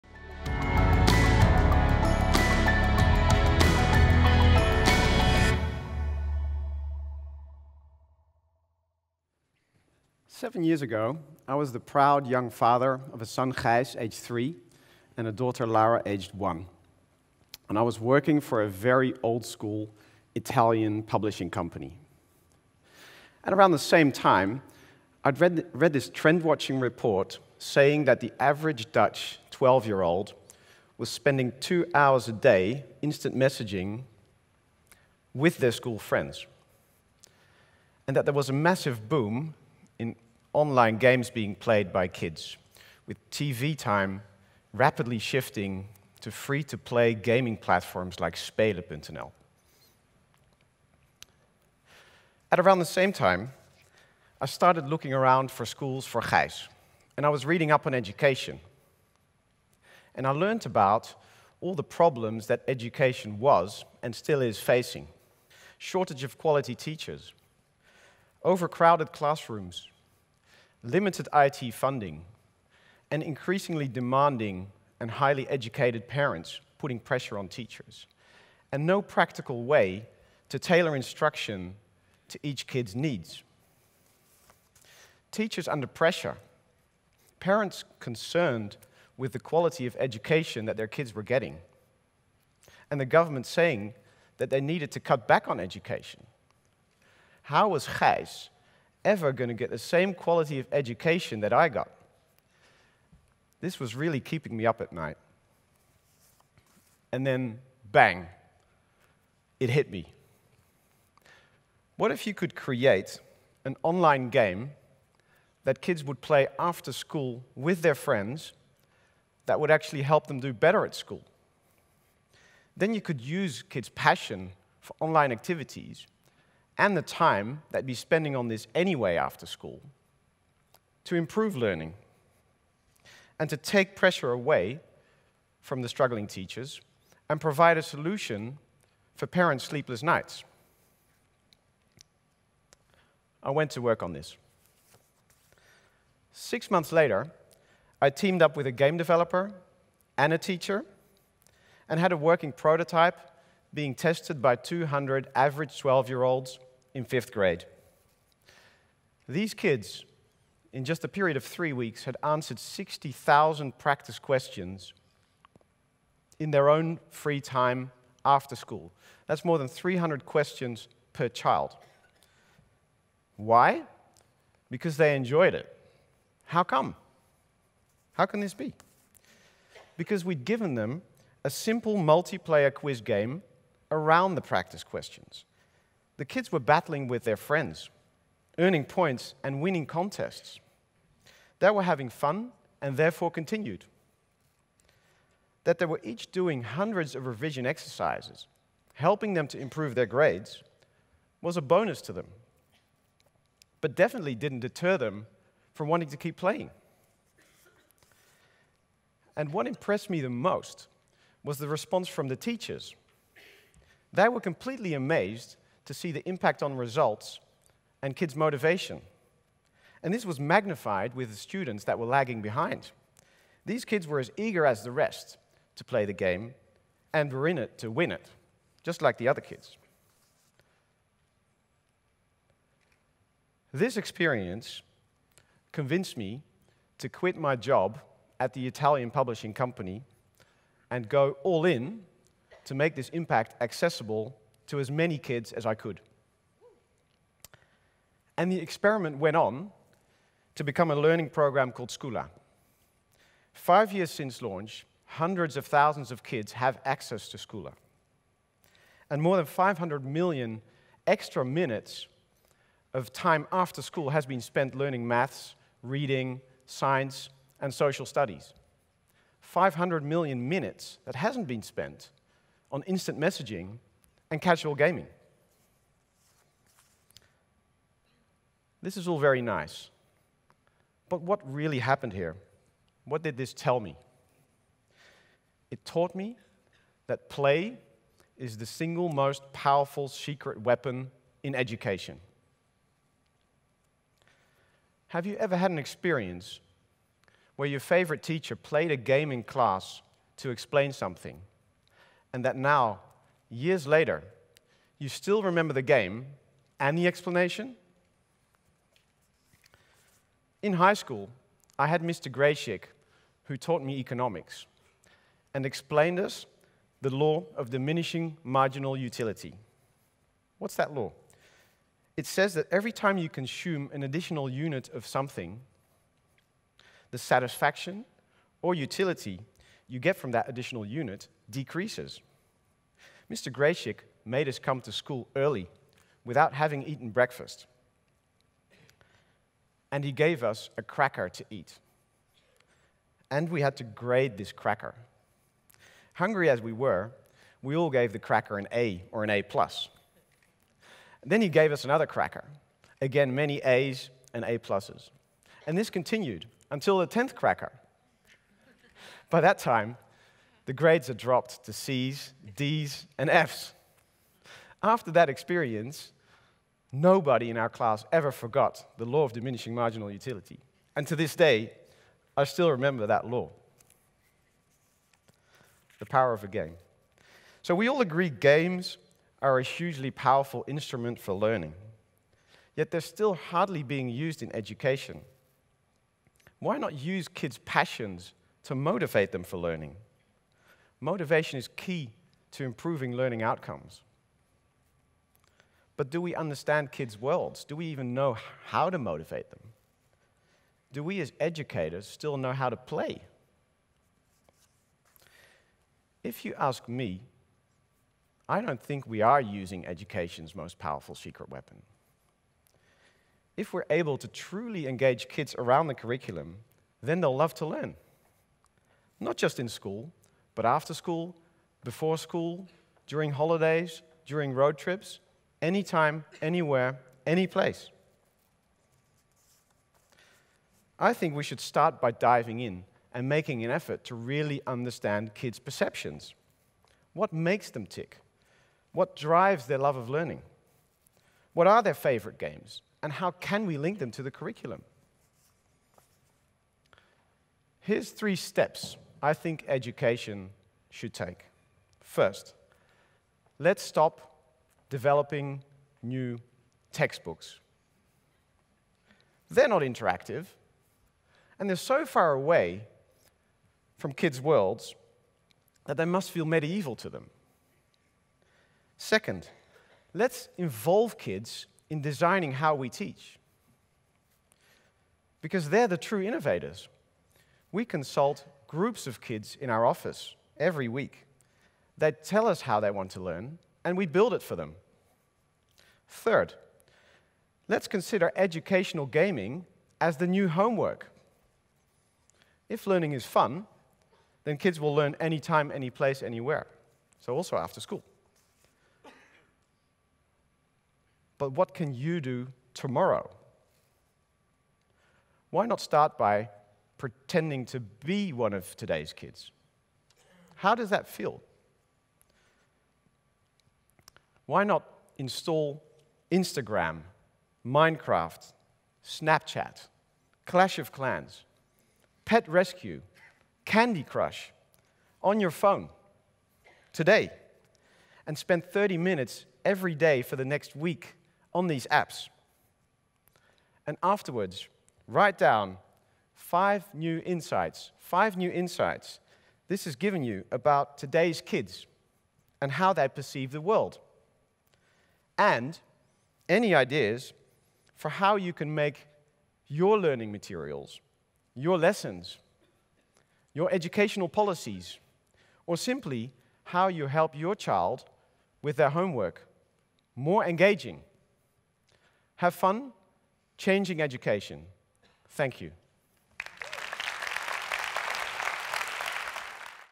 How I stumbled on education’s most powerful weapon: Play | André Haardt | TEDxAmsterdamED
Kids want screen time. Many adults and parents fear all the energy spent on iPads, videos, and other media. But what if it made sense to embrace kids’ love of new technologies, while also helping them learn? In this thought-provoking talk, Andre Haardt explains how he stumbled upon a fun way to get kids to enjoy learning.

In 2008, Andre was working for an Italian printing company and during that time he read an article about how most children spend their time after school - two hours of instant messaging and playing games. And this was even before Facebook and social media entered our lives. Andre came up with an idea: why don’t we create a game that children can play after school, a game that actually supports their learning? Now, Andre owns a company that creates engaging games for children that they can play after school. Instead of being on Facebook for two hours, or chatting away on social media, kids are given the opportunity to play and learn.